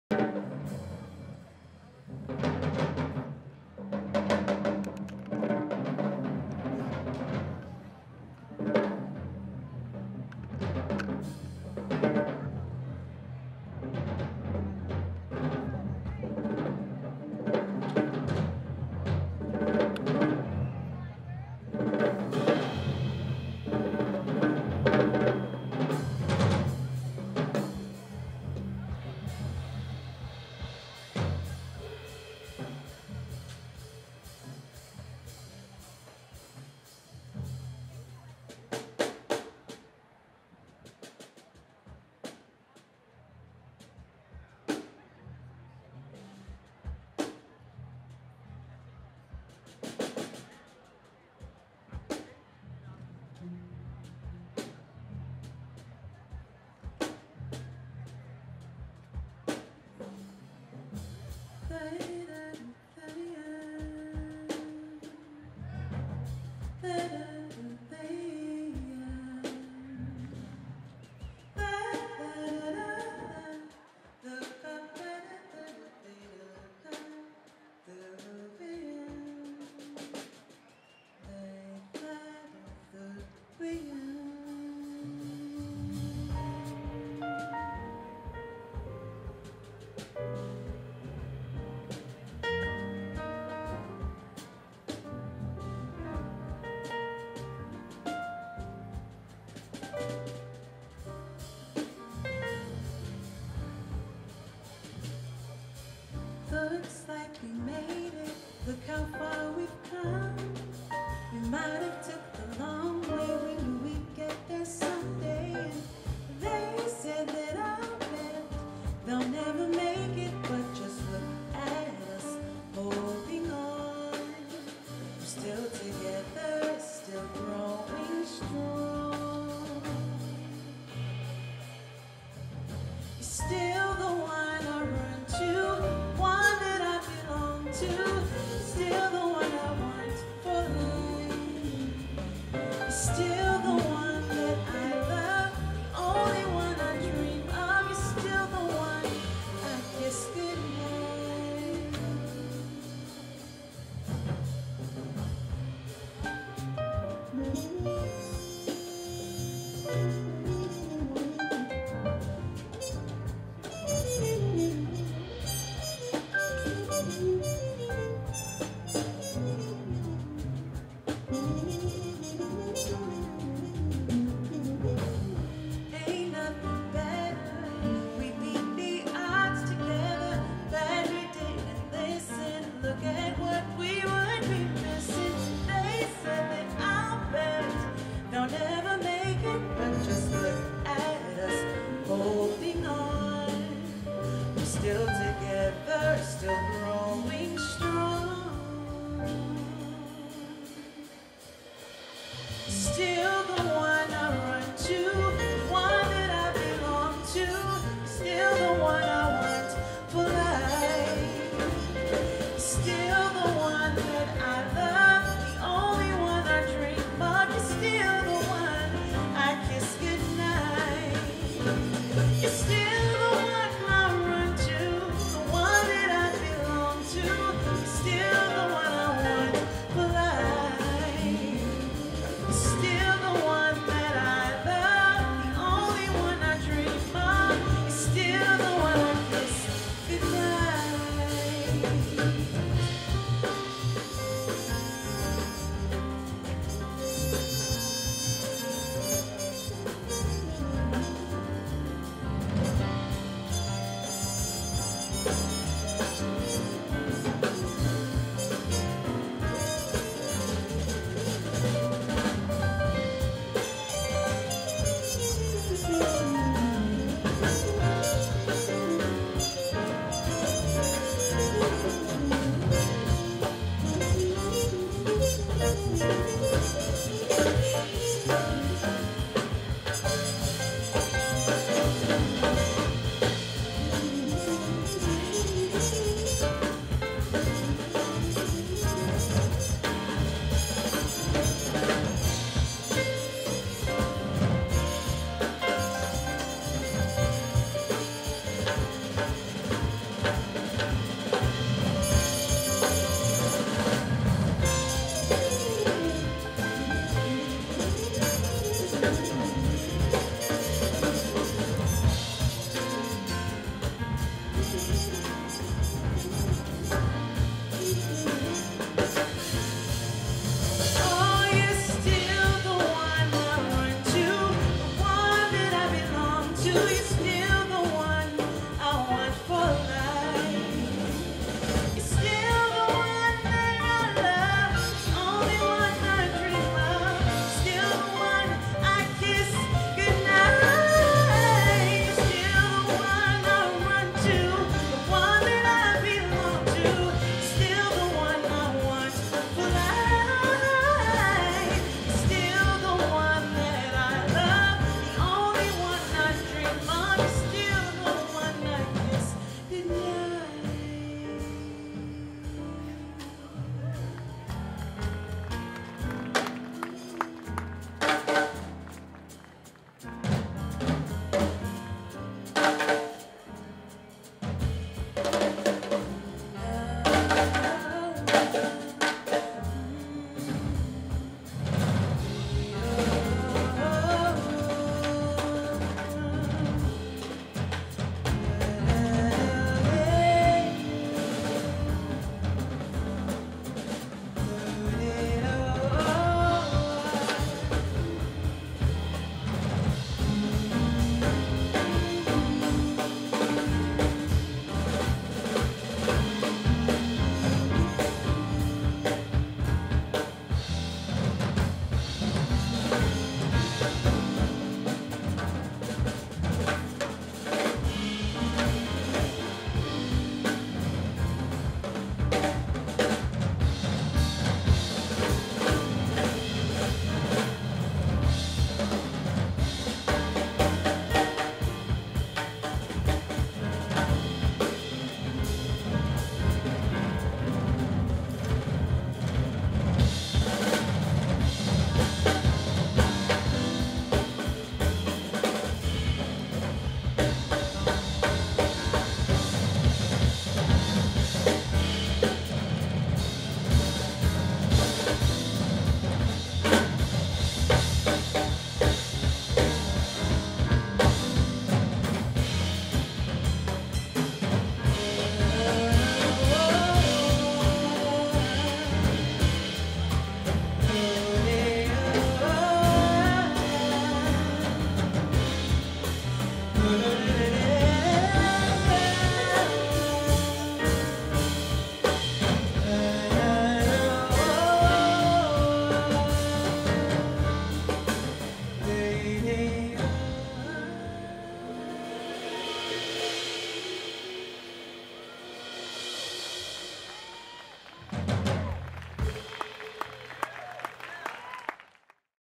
Otis Brown III @ The 2015 Atlanta Jazz Festival
Otis Brown III @ The 2015  Atlanta Jazz Festival. More live video can be seen on the Channel: Visual Rhythms from the Chill Vibe Experience